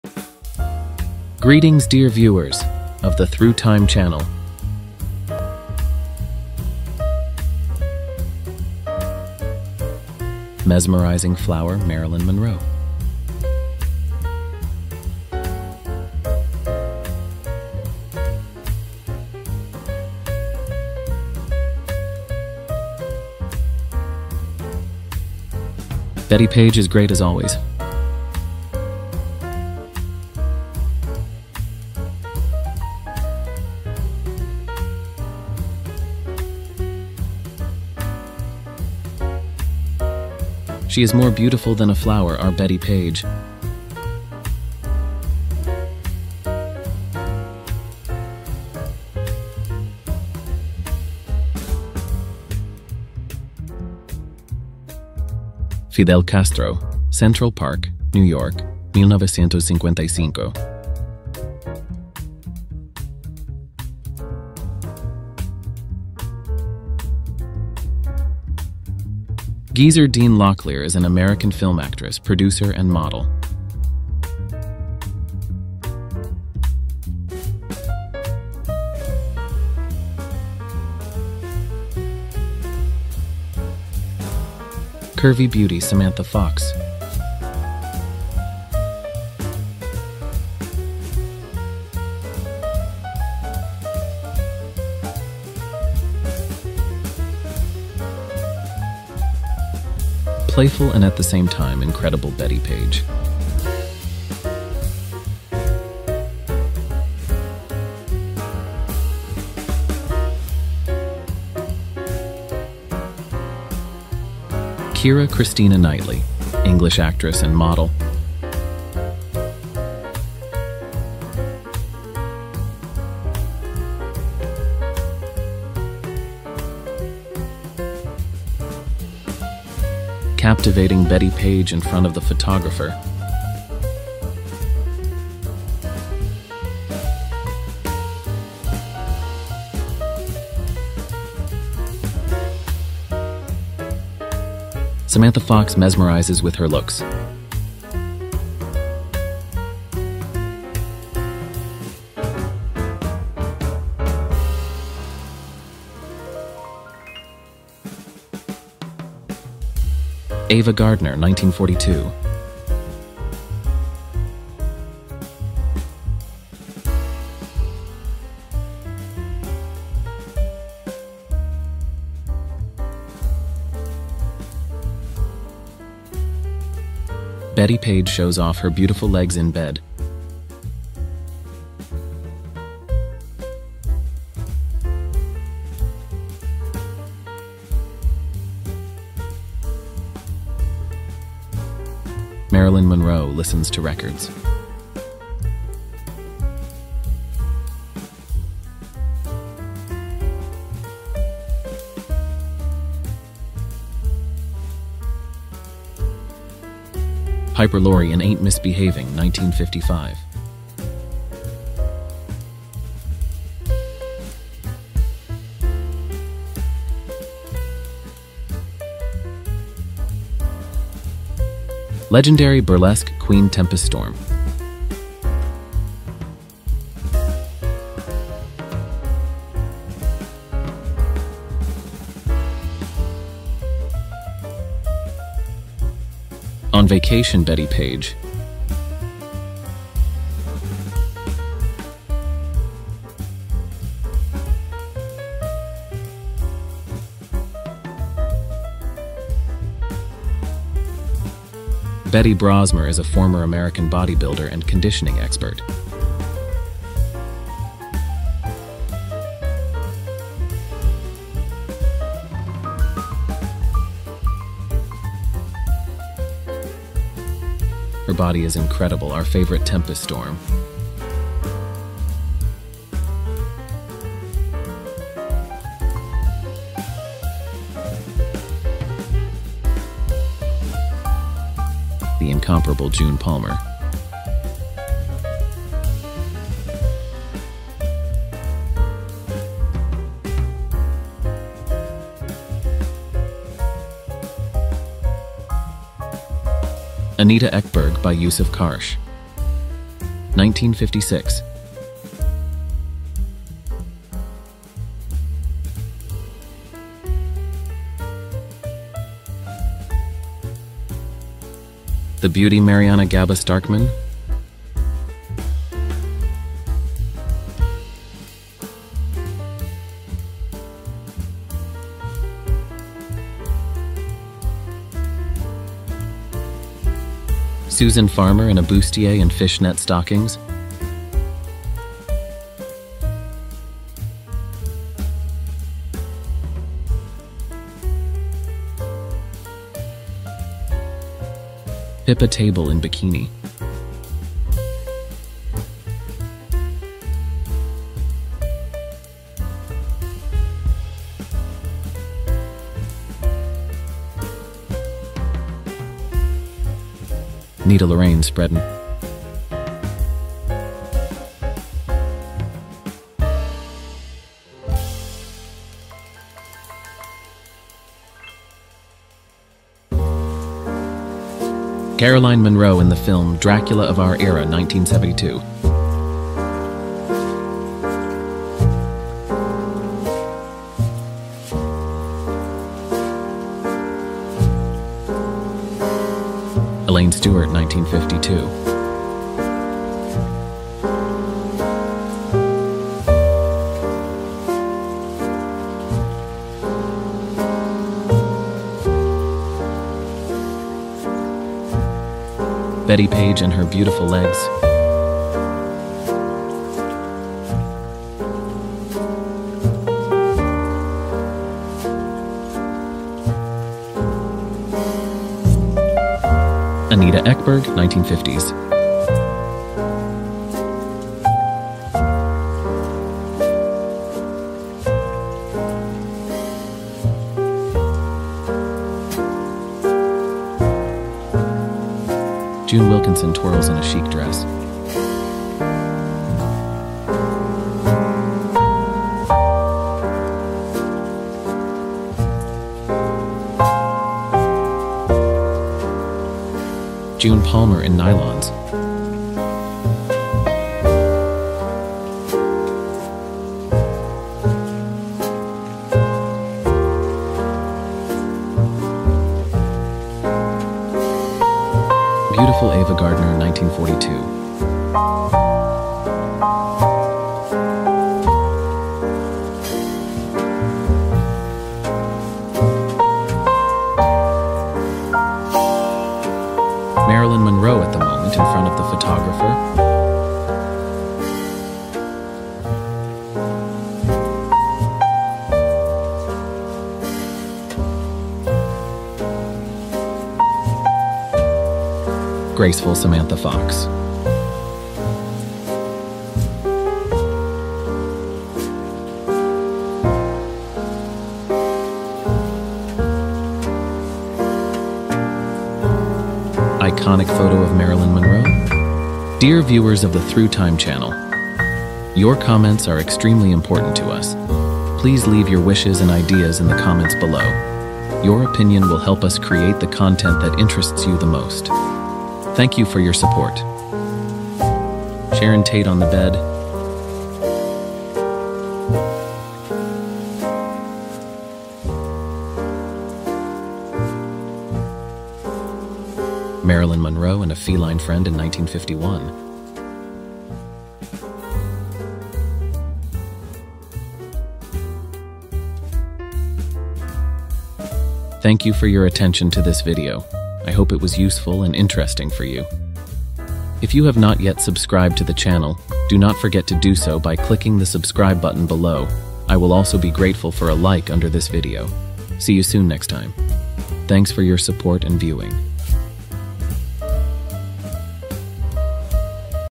BEAUTIFUL SELECTION OF VINTAGE HISTORICAL PHOTOGRAPHS
The THROUGH TIME channel offers you a unique opportunity to immerse yourself in the aesthetics of past eras.
 In this video "Bettie Page BEAUTIFUL SELECTION OF VINTAGE HISTORICAL PHOTOGRAPHS" we pay tribute to the legendary model of the 50s, Bettie Page, by showing a collection of unrivaled vintage photographs. The luxury and elegance of a bygone era come to life through this unique archive, which offers a glimpse into the beauty and style of decades past.